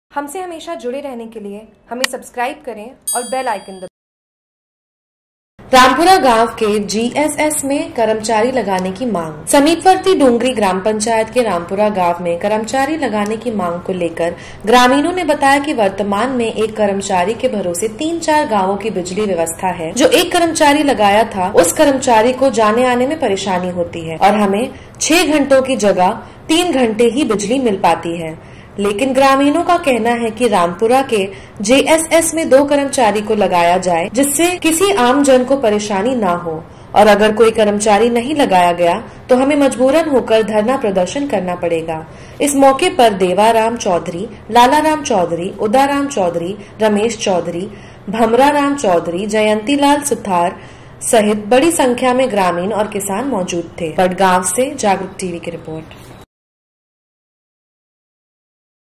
JAGRUK TV :बडगांव : रामपुरा गाव के जीएसएस में कर्मचारी लगाने की मांग
बडगांव। समीपवर्ती डूंगरी ग्राम पंचायत के रामपुरा गाव में कर्मचारी लगाने की मांग को लेकर ग्रामीणों ने बताया कि वर्तमान में एक कर्मचारी के भरोसे तीन-चार गावों की बिजली व्यवस्था है। जो एक कर्मचारी लगाया गया था जो उस कर्मचारी को कई जाने आने में परेशानी होती है। और हमे 6 घण्टो की जगह 3 घण्टे ही बिजली मिलपाती है।